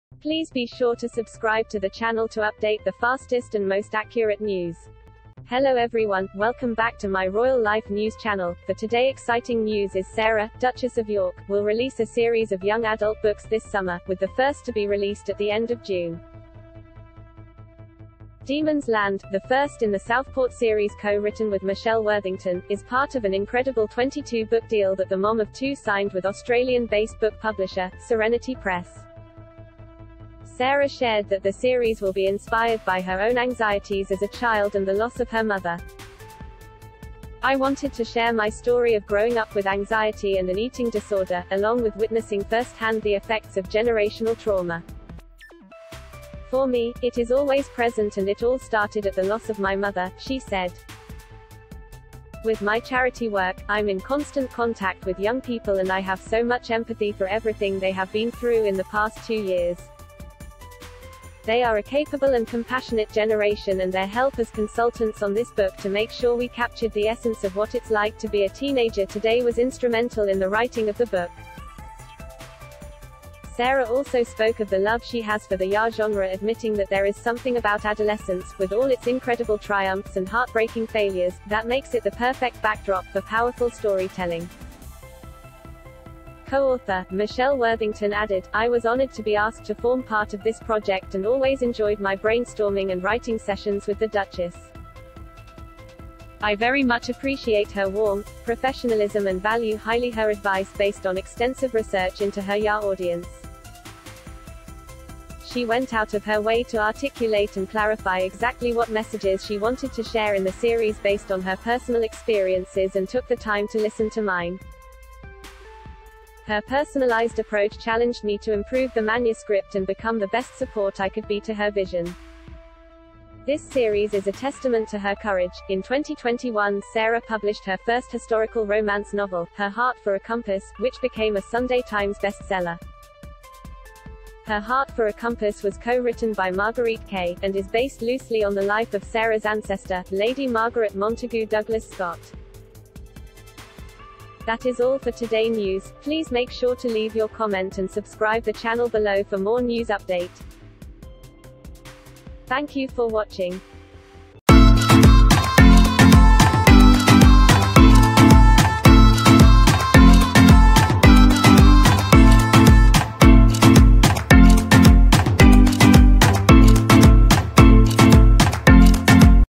Sarah Ferguson to pen three YA novels as part of 22 book deal
The Duchess of York's first book will be released 30 June